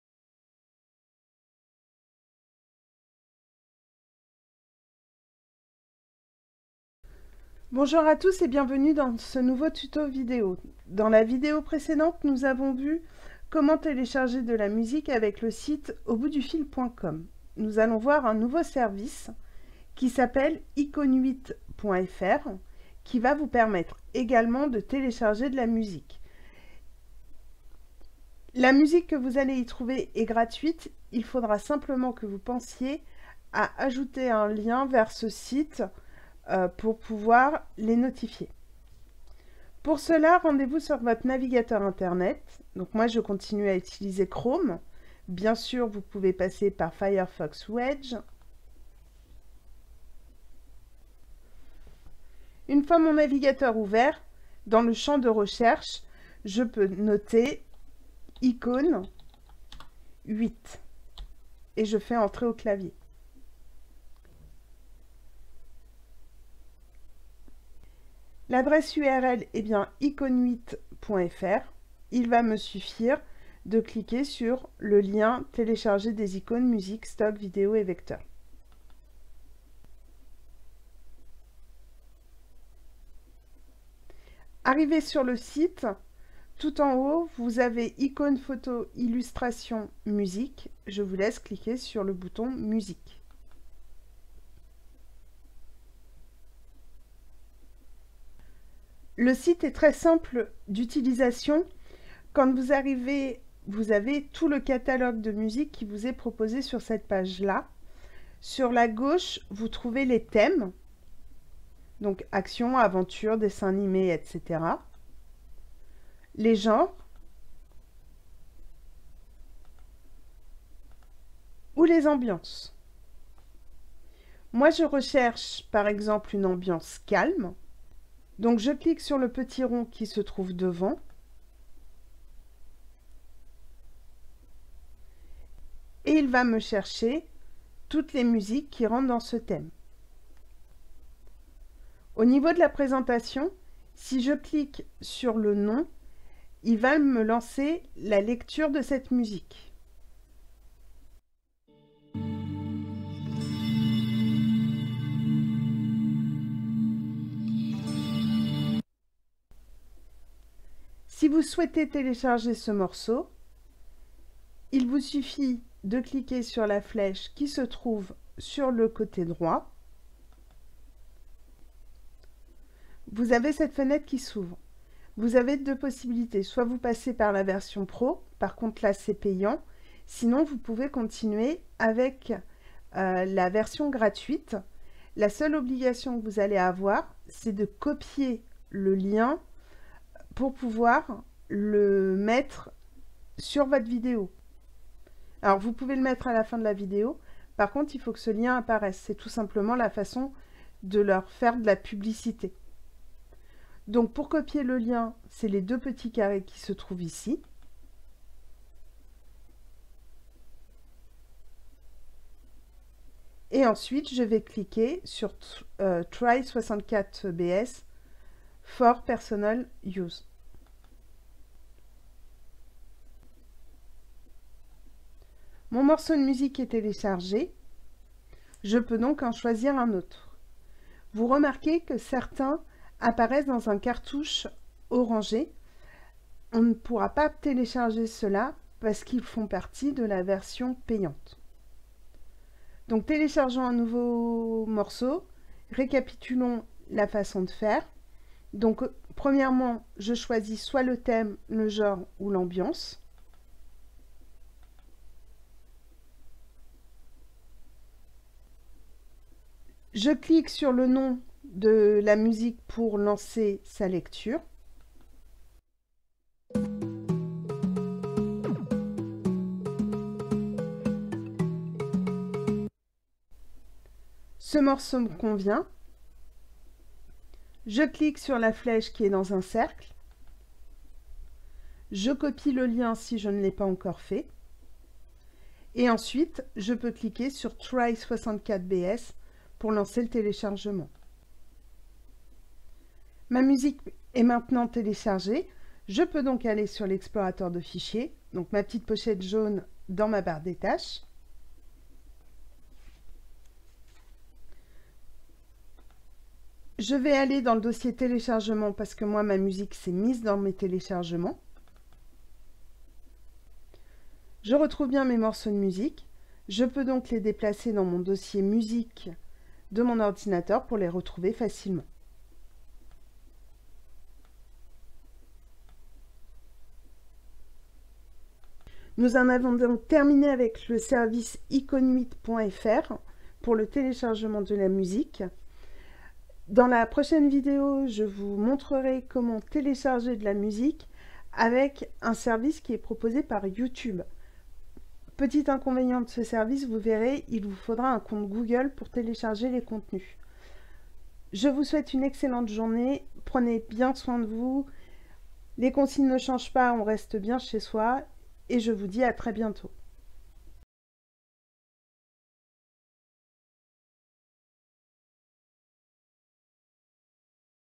Télécharger du contenu libre de droit - Partie 6 : Icones8.fr - Télécharger de la musique
vous pouvez télécharger de la musique libre de droit. La seule obligation que vous avez pour l'utiliser et de notifier le site en copiant le lien qui renvoie sur leur page.